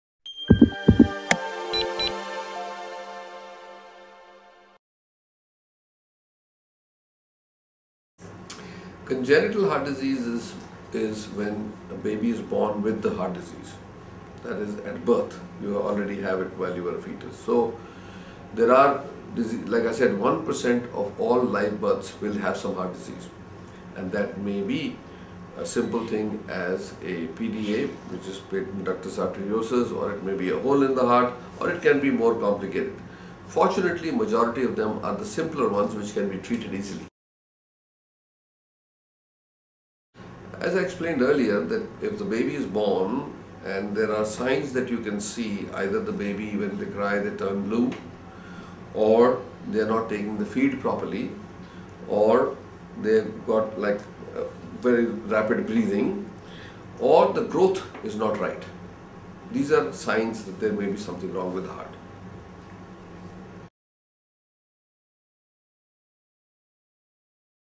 Dr. Naresh Trehan of Medanta Hospital on Congenital Heart Disease
In this video, Dr Naresh Trehan, reputed Cardio Thoracic & Vascular Suregon explains what a Congenital Heart Disease is and how it can be detected at an early age itself. He clearly explains the symptoms among children that can help people identify cause of worry and need for a check up.